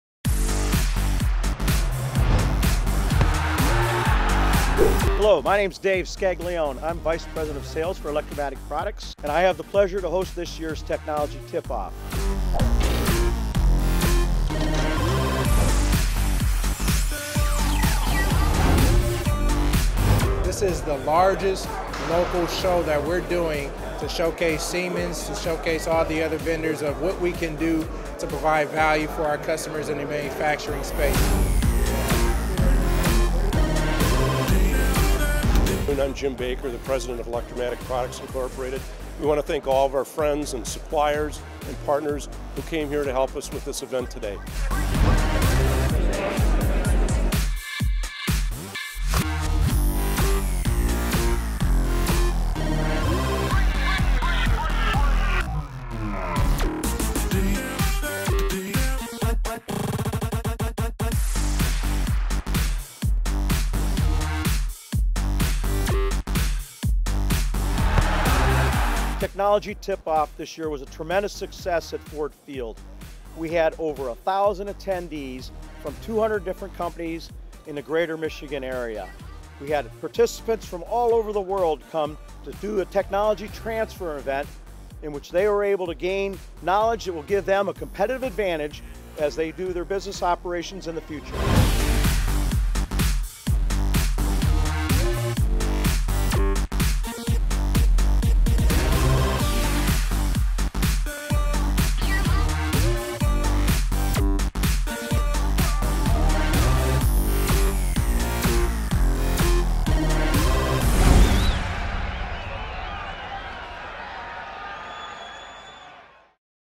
Electro-Matic Products Industrial Automation Technology Event
Electro-Matic-\ Products hosted the 2012 Industrial Automation Technology Transfer Event in Detroit, MI at Ford Field. There were over 1000 attendees from more than 200 companies from the greater Michigan area. Participants from all over the world came to the Technology Transfer Event, in which they were able to gain knowledge that would give them a competitive advantage as they do their business operations in the future. Siemens, Rittal, Turck, Eplan and Electro-Matic hosted the 2012 Technology Transfer event featuring new Industrial Automation Technology at Ford Field in Detroit, MI. The event brought in industry experts from all over the world who showcased their best of class technology to attendees. The many seminars offered and products demonstrated were proven to be the latest in automation trends.